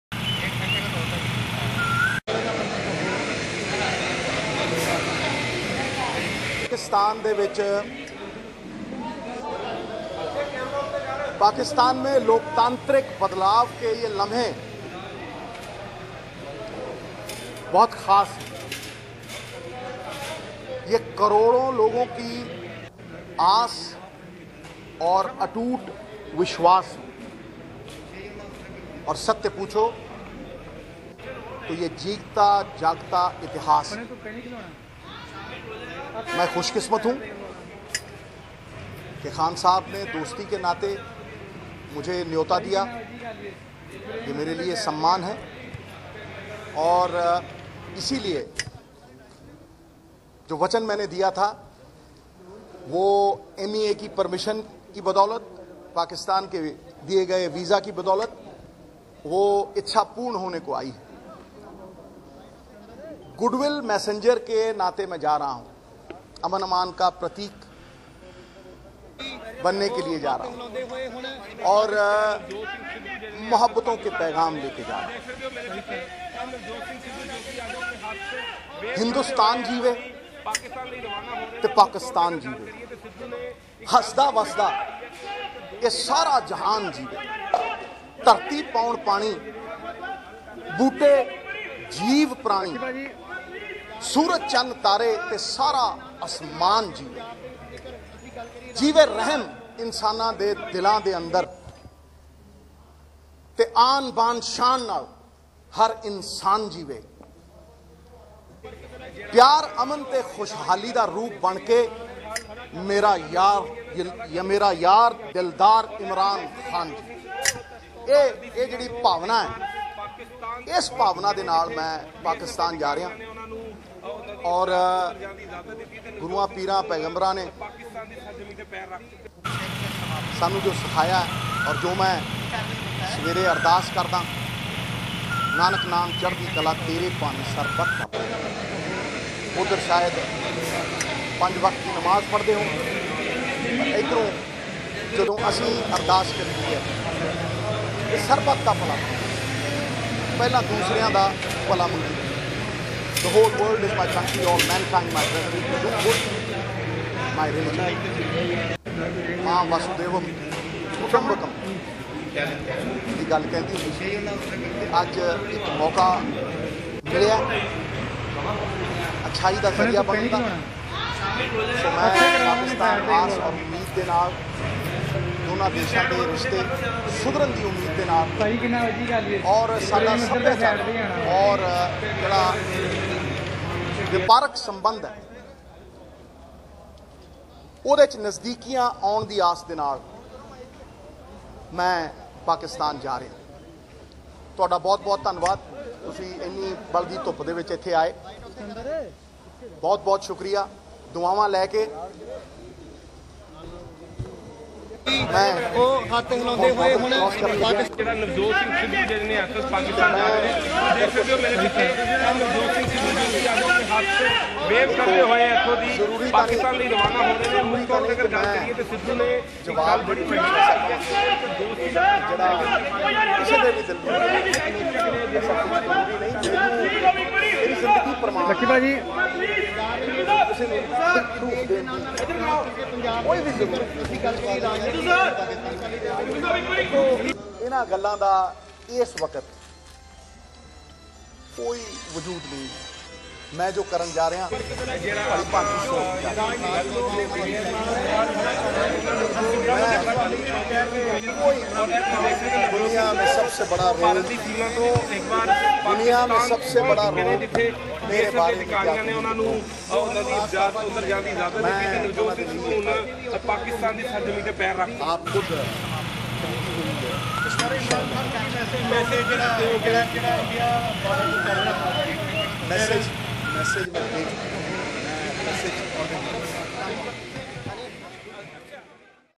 Navjot Singh Sidhu Arrives For Imran Khan's Oath Taking Ceremony In Pak
Navjot Singh Sidhu arrived Lahore via the Wagah border from where he will travel to Islamabad to attend Imran Khan's oath-taking ceremony scheduled for tomorrow.